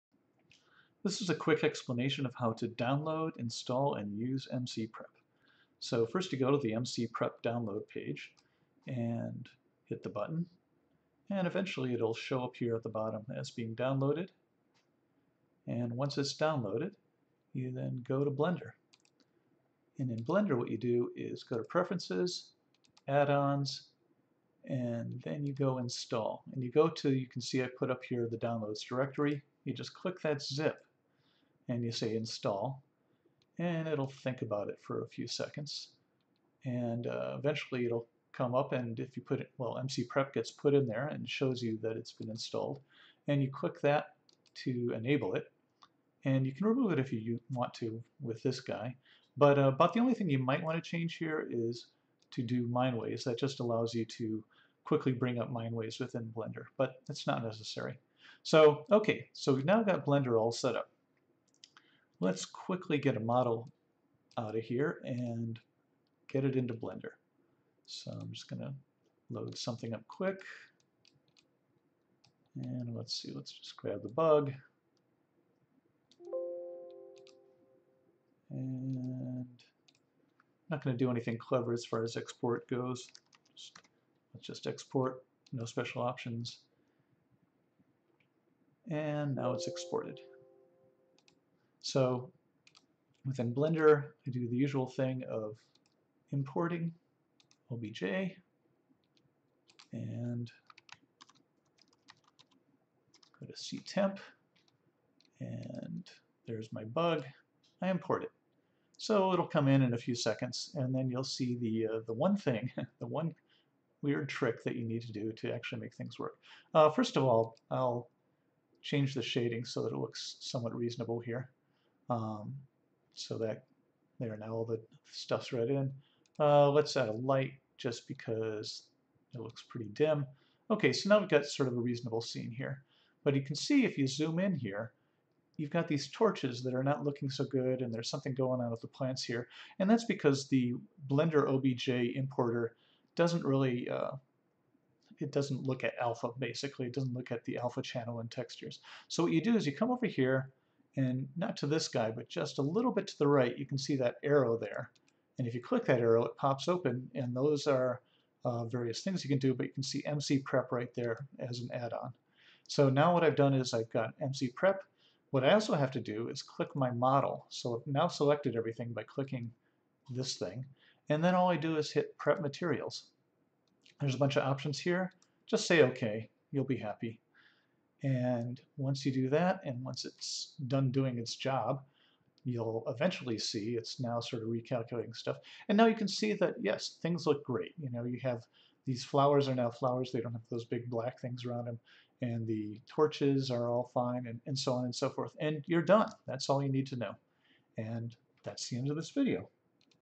MCprep download, install, and use in Blender
How to install MCprep, which nicely processes materials from Mineways exports for use in Blender. The written summary: download the MCprep zip (link below) and in Blender: Edit, Preferences, Add-ons, Install and point at this zip. Once installed, click the box to the left of "Object: MCprep" in the Add-ons dialog. Done!

One comment from the creator of MCprep: at 1:24, it’s helpful for Blender/MCprep to also tick the box for “Split by block type” (and, if someone wants animated textures like water, to also hit that “Export tiles for textures to..” option). But, only critical if they are wanting to use “mesh swap” or do “Swap textures” after the fact inside of Blender.